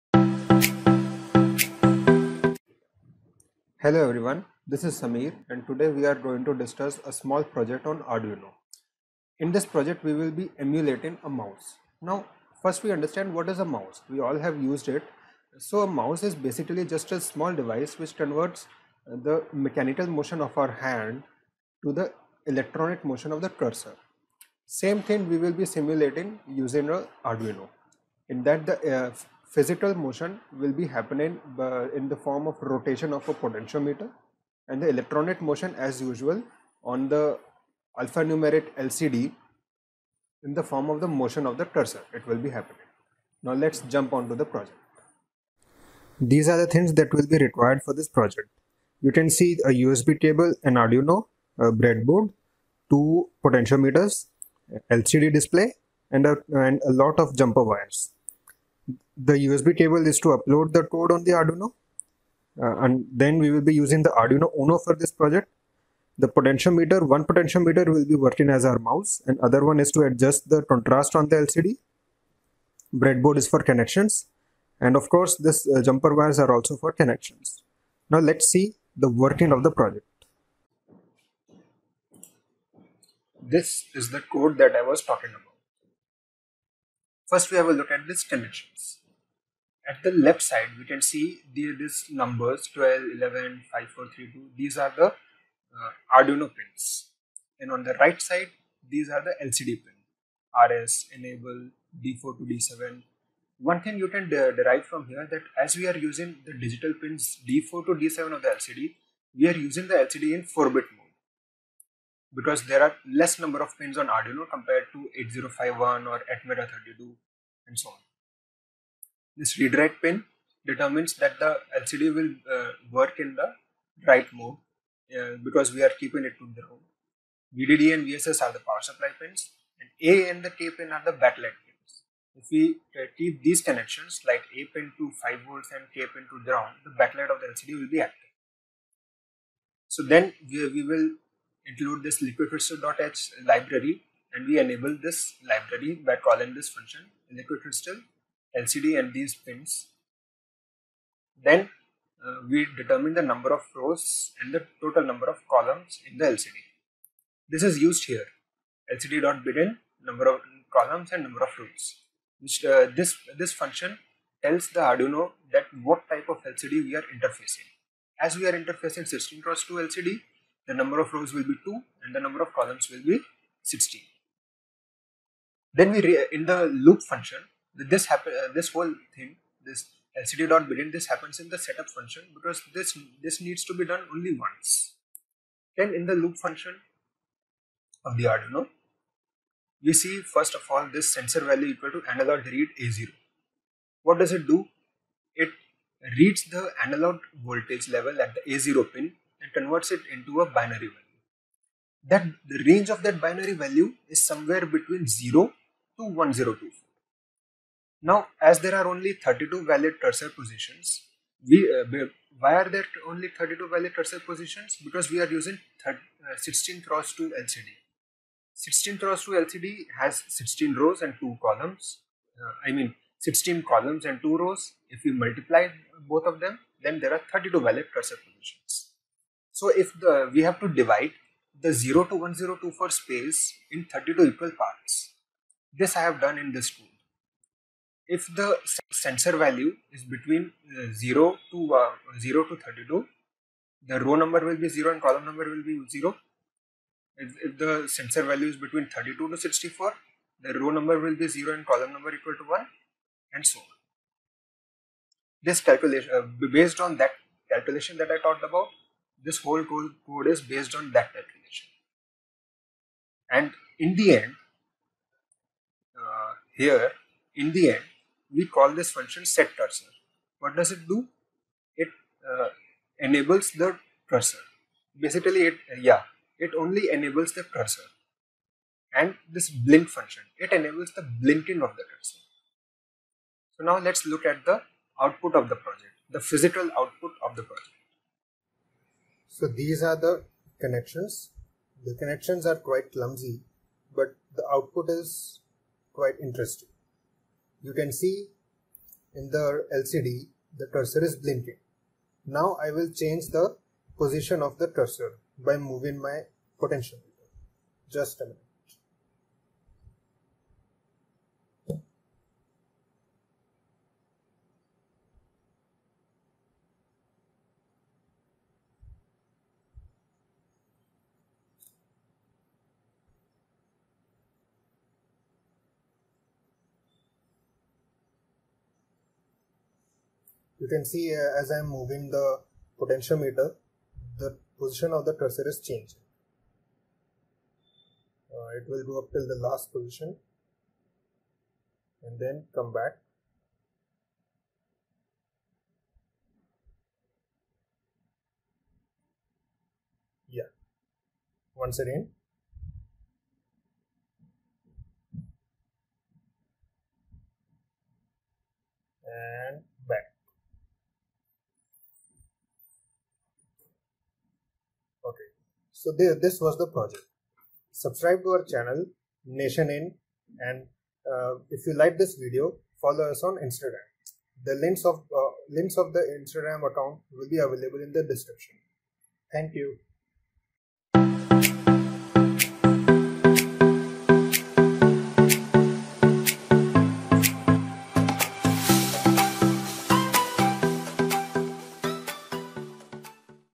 Nation Innovation : How to make Mouse using Arduino UNO? | DIY New Project | 2020
Title: How to make Mouse using Arduino? | New Project | 2020

Topics:
✅  How to make a mouse using the Arduino UNO?
✅  DIY Projects
✅  Project Working

👨‍🏫: Mr Sameer Narang
📺: Nation Innovation

Like👍 ll comments📝 II Share📢

➡️Keep Supporting🙏